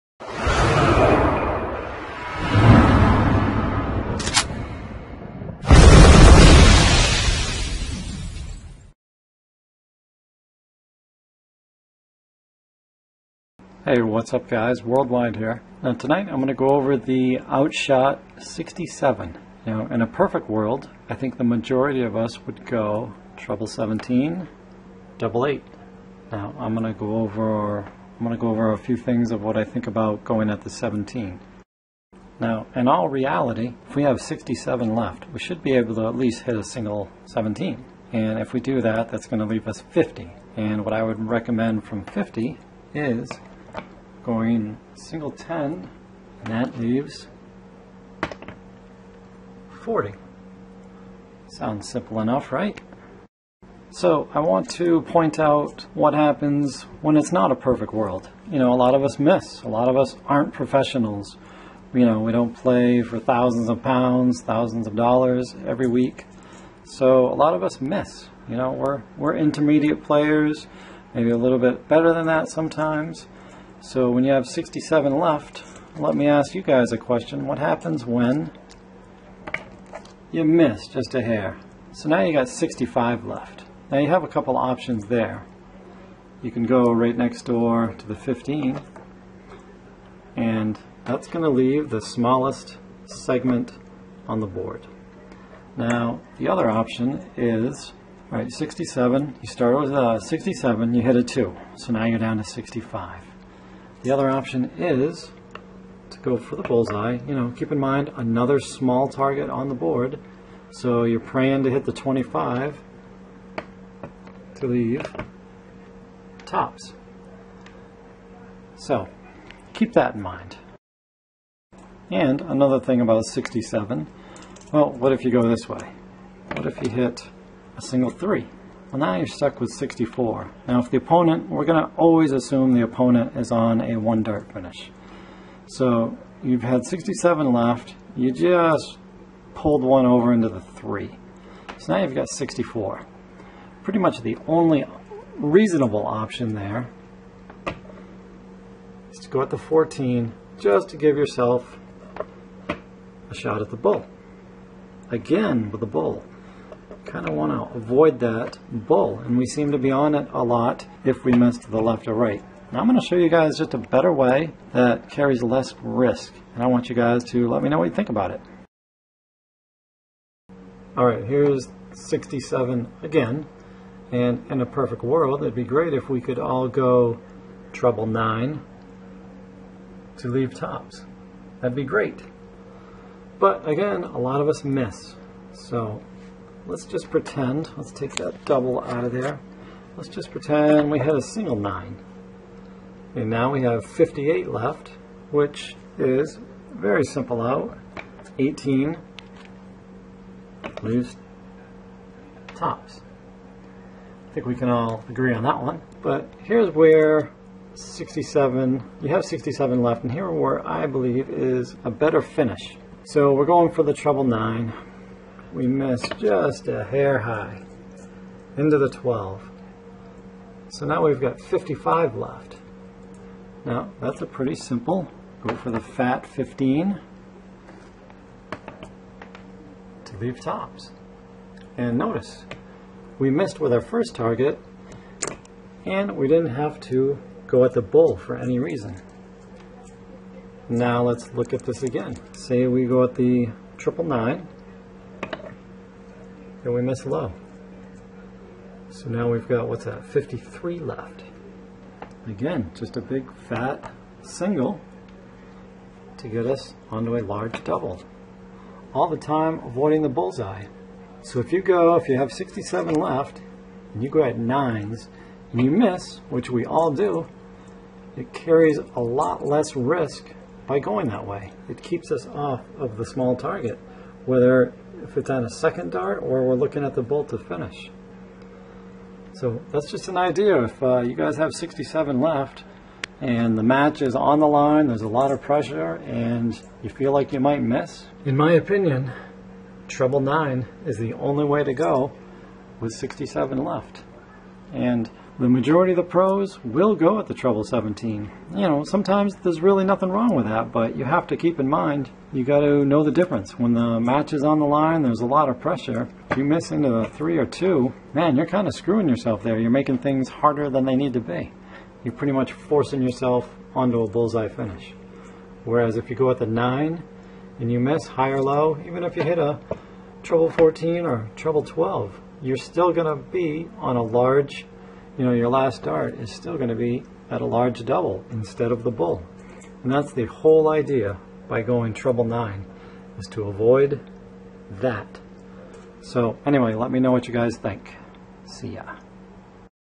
67 Checkout - 17's is Common But Is There A Better Way?
I think most of us go 17's with 67 left. But there's a better/safer/less risky option.